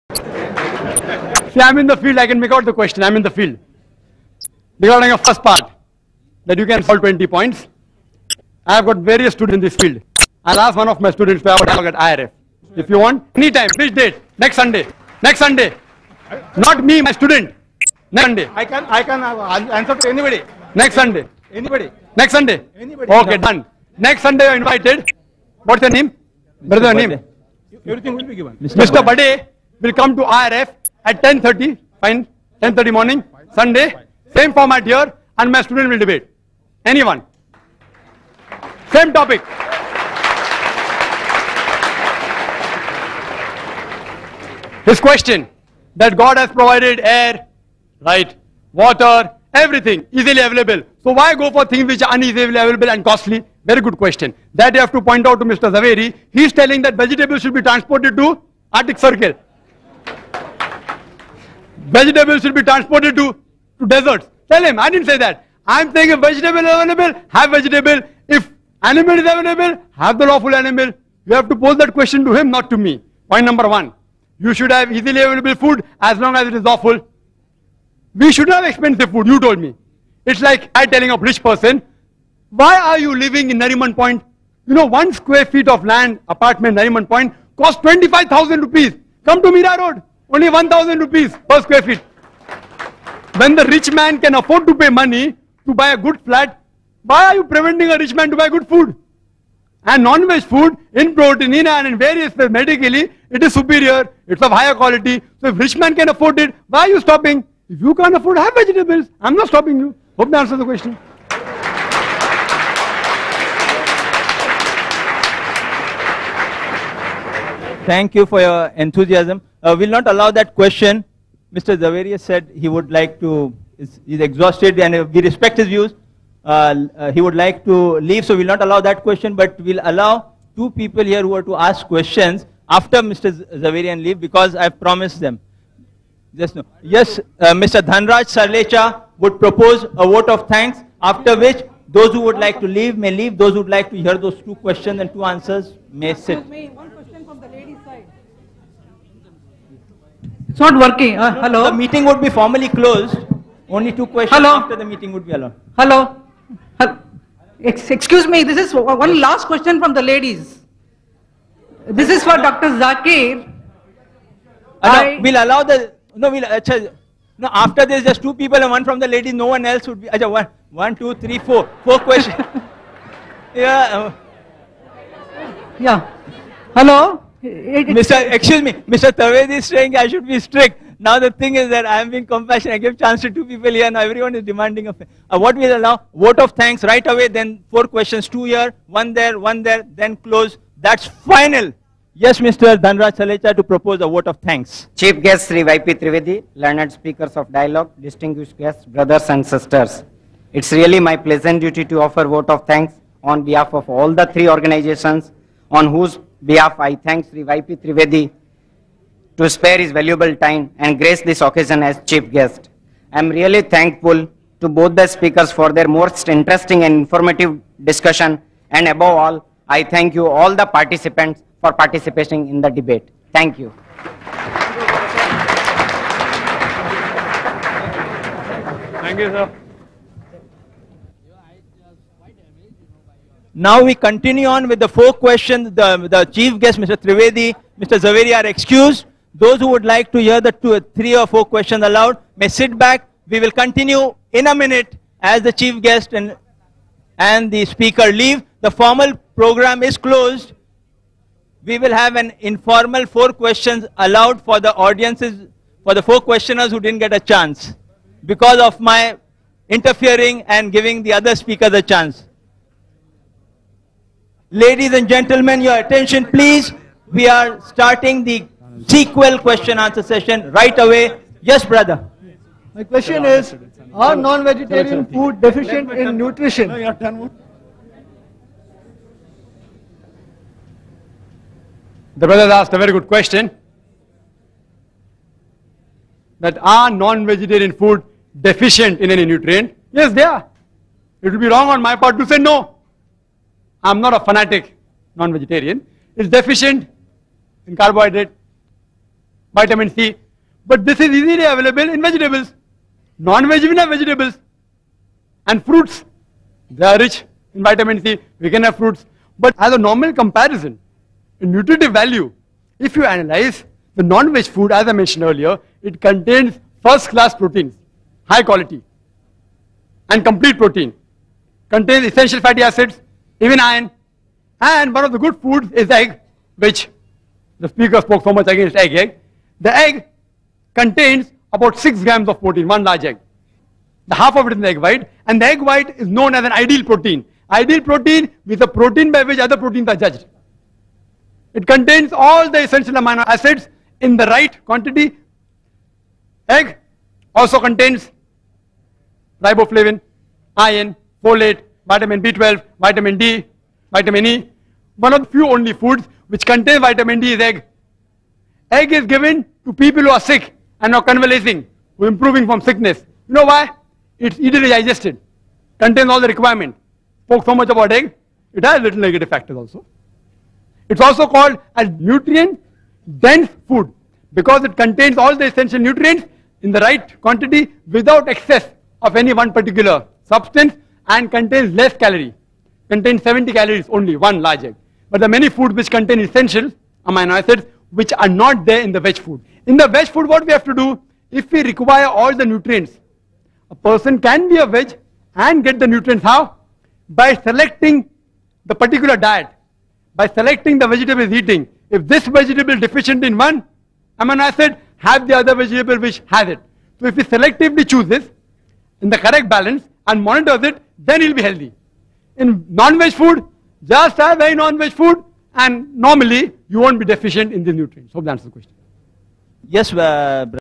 Dr. Zakir Naik - Is Non-Vegetarian Food Permitted or Prohibited for Mankind Part 29 of 30
Zakir Naik